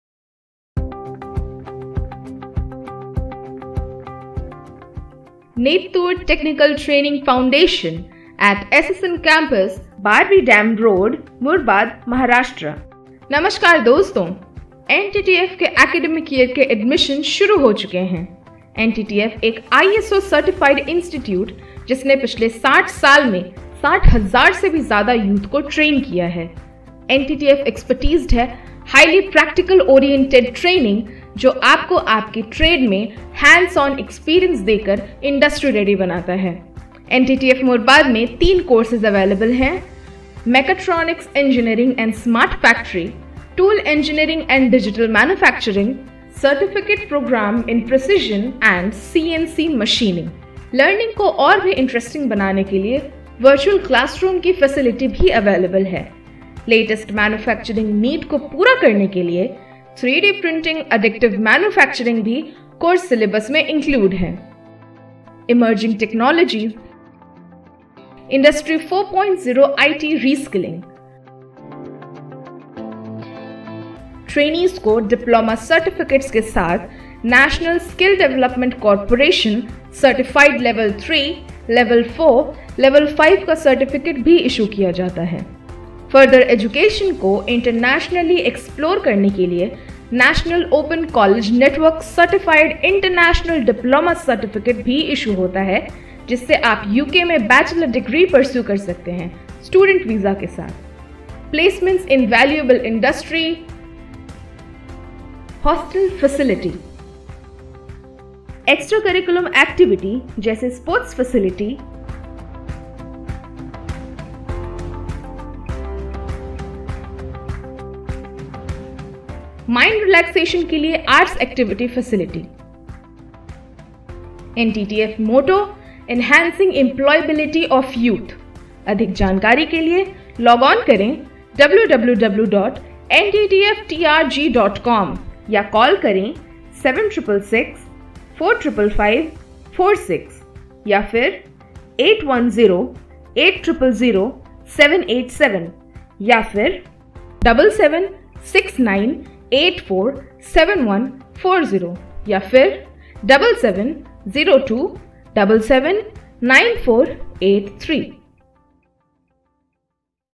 NTTF - NETTUR TECHNICAL TRAINING FOUNDATION
Dr Heinrich Hellstern, Director of HEKS (a Swiss NGO), through his noble gesture, set the ball rolling for opening a Technical Training institute at Nettur, Tellicherry (Kerala), in 1959. Nettur Technical Training Foundation was formed in 1963. NTTF is the repository of active technical support and knowledge transfer from Swiss industry and training institutions. The seed was sown 50 years ago - the decision to start tool and die-making courses at Tellicherry - has grown into a productive plant, surpassing even the most optimistic of expectations. The foundation has successfully developed itself and promoted technical training to suit the needs of the society, all across India.